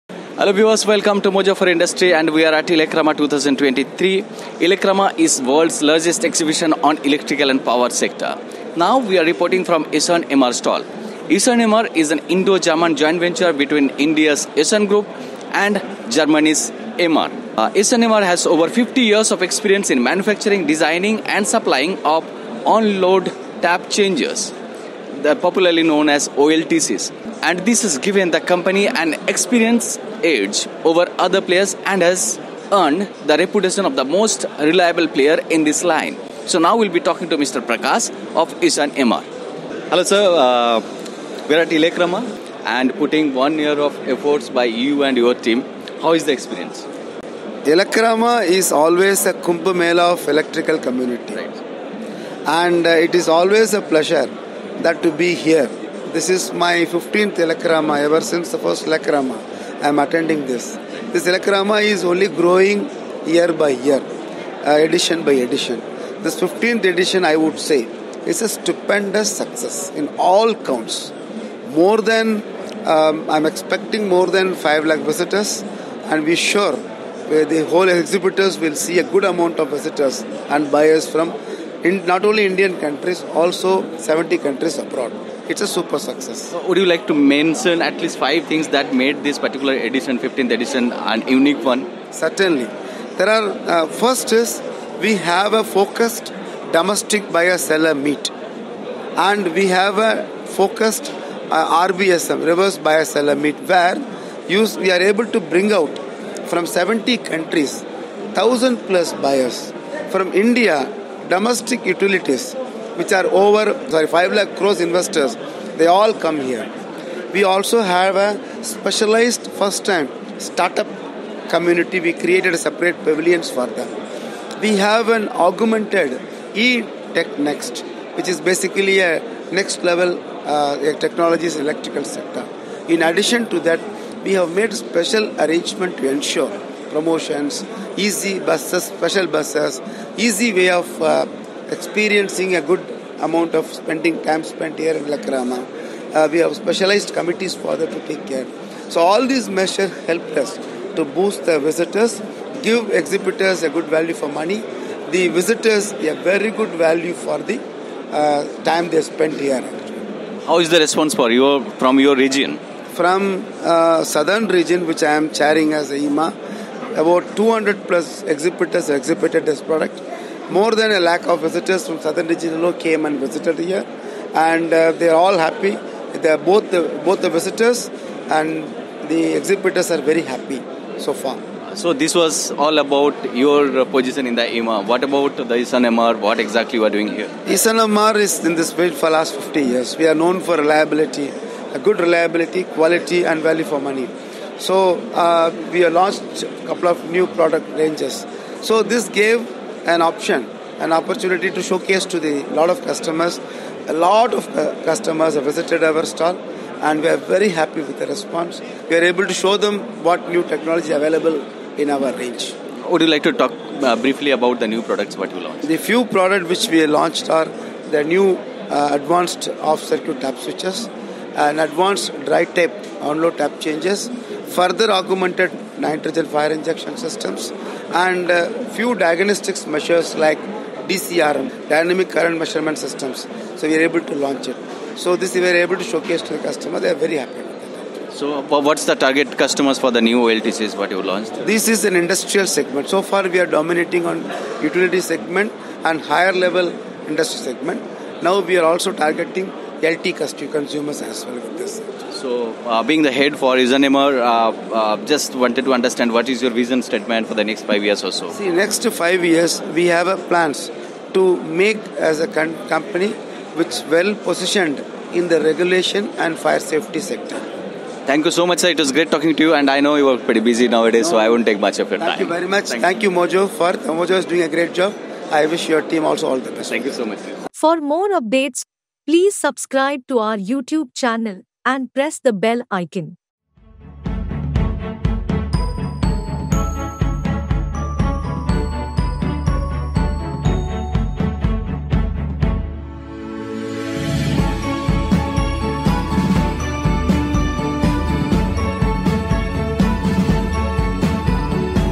EASUN - MR launches advanced off-circuit tap switches | ELECRAMA 2023
According to Prakash R, Vice President, Easun MR Tap Changers Private Limited , ELECRAMA is always a Kumbh Mela of electrical community and it is always a pleasure to be there. He adds, "ELECRAMA is only growing edition by edition and the 15th edition (ELECRAMA 2023) was a stupendous success in all counts – with more than 5 lakh visitors attending the event from 70 countries across the world.”

Mr. Prakash is also the Chairman of Southern Region at Indian Electrical & Electronics Manufacturers'​ Association (IEEMA). Talking about the participation from Southern region, he said, “About 200-plus exhibitors exhibited their product. More than a lakh of visitors from Southern Region have visited the event.”

EASUN - MR is in this field for the last 50 years and is known for reliable, quality and value for money products. At the ELECRAMA 2023, the company has launched a host of new products including advanced off-circuit tap switches, advanced dry type on-load tap changers, further augmented nitrogen fire injection systems, and a few diagnostics measures like DCRM (dynamic current measurement systems).

“Till now, we are dominating on utilities and higher level industry segment. With the launch of new OLTC, we will be targeting LT consumers segment as well,” said Mr. Prakash.

Sharing his vision statement for Indian market, he said, “In next 5 years, we have plans to be a company which is well-positioned in the regulation and fire safety sector.”

WATCH HIS FULL INTERVIEW!